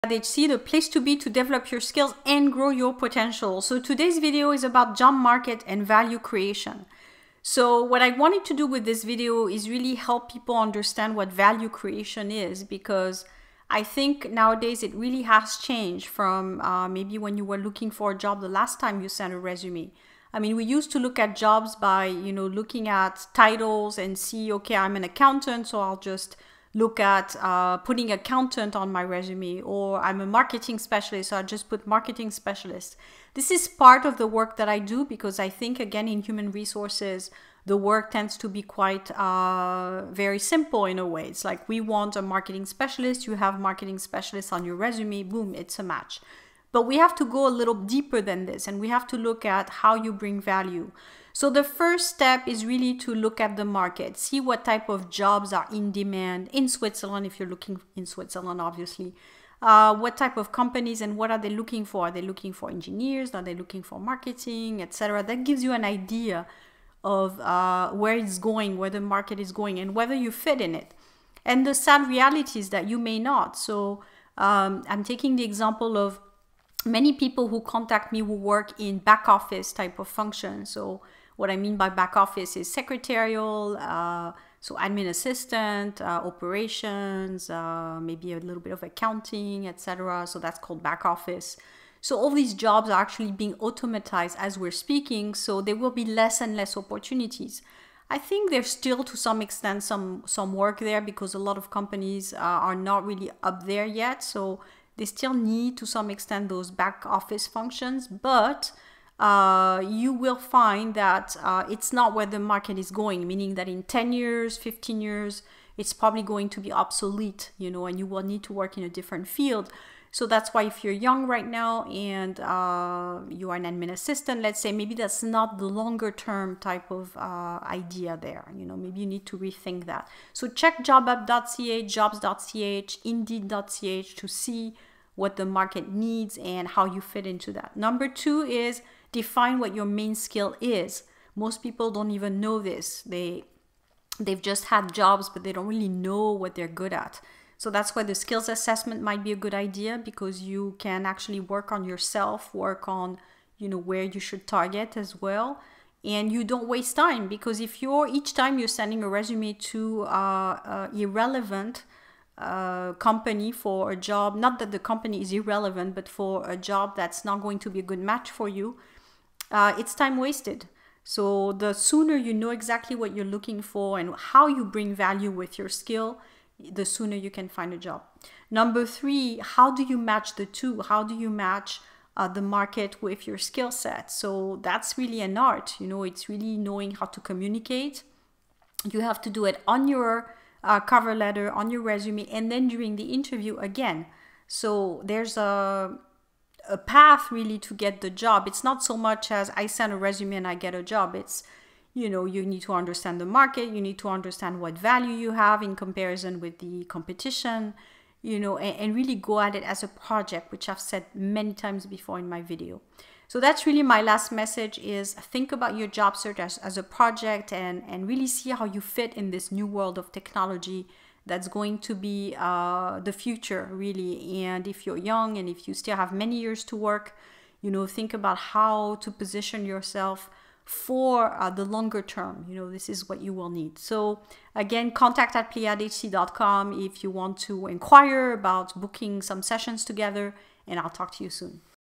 How to bring value to the job market
Today, I give you 3 tips on how to bring value to the job market

- Study the market (jobup.ch, jobs.ch, indeed.ch)
- Define what your main skill is
- Merge the above two components